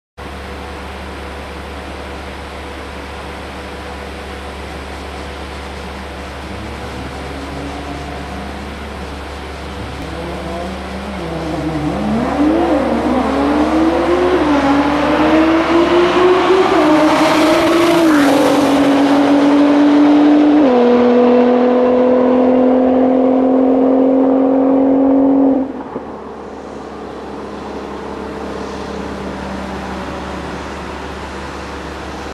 BAHRAIN , AL KHAJA GTR RUNS 10.91 @ 204.67
MOHAMED AL KHAJA GTR RUNS 10.91 @ 204.67 WITH MINOR BOLT- ONS AND STOCK WHEELS , BUILT AND TUNNED BY E.KANOO